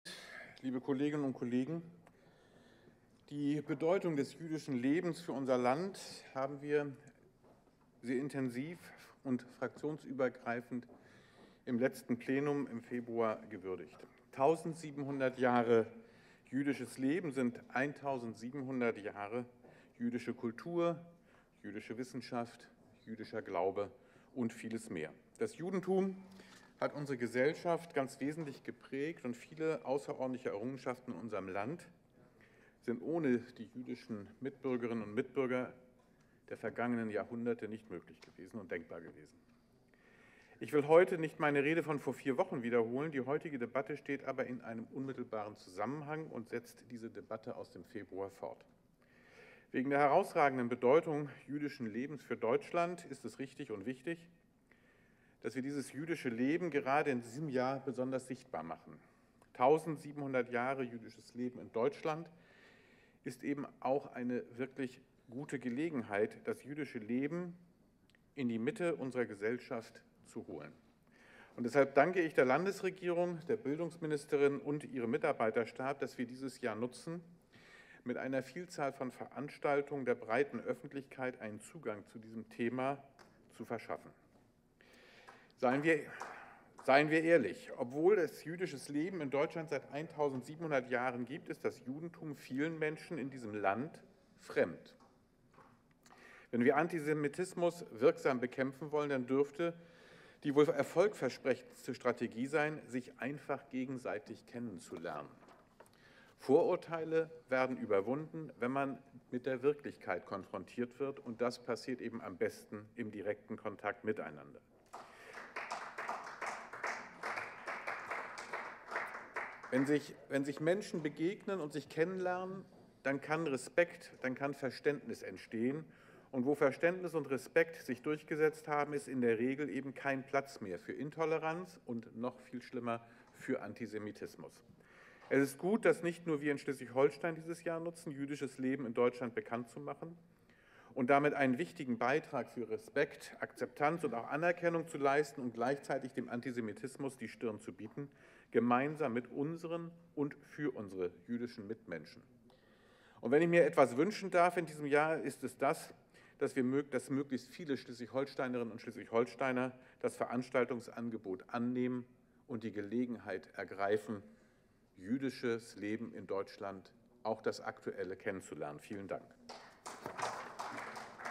Jan Marcus Rossa zu TOP13:Planungen der Landesregierung zu 1700 Jahre jüdisches Leben in Deutschland
,,Die Bedeutung des jüdischen Lebens für unser Land haben wir im letzten Plenum fraktionsübergreifend gewürdigt. 1.700 Jahre jüdisches Leben sind
1.700 Jahre jüdische Kultur, jüdische Wissenschaft und vieles mehr. Das Ju- dentum hat unsere Gesellschaft ganz wesentlich geprägt und viele außerordentliche Errungenschaften in unserem Land sind ohne die jüdischen Mit- bürgerinnen und Mitbürger nicht denkbar.

Ich will heute nicht meine Rede von vor vier Wochen wiederholen. Die heu- tige Debatte steht aber in unmittelbarem Zusammenhang mit der Debatte im Februar. Wegen der herausragenden Bedeutung jüdischen Lebens für Deutschland ist es richtig und wichtig, dass wir dieses jüdische Leben gera- de in diesem Jahr besonders sichtbar machen. ,1.700 Jahre jüdisches Leben in Deutschland` ist eine hervorragende Gelegenheit, das jüdische Leben in die Mitte unserer Gesellschaft zu holen. Und deshalb danke ich der Landes- regierung, der Bildungsministerin und ihrem Mitarbeiterstab, dass wir dieses Jahr nutzen, mit einer Vielzahl von Veranstaltungen der breiteren Öffentlich- keit einen Zugang zu diesem Thema ,jüdisches Leben in Deutschland` zu verschaffen.

Seien wir ehrlich: Obwohl es jüdisches Leben in Deutschland seit 1.700 Jahren gibt, ist das Judentum vielen Menschen in diesem Land fremd. Wenn wir Antisemitismus wirksam bekämpfen wollen, dann dürfte die wohl erfolgver- sprechendste Strategie sein, sich einfach einmal gegenseitig kennenzuler- nen. Vorurteile werden überwunden, wenn man mit der Wirklichkeit konfrontiert wird. Wenn sich Menschen begegnen und sich kennenlernen, ent- stehen Verständnis und Respekt.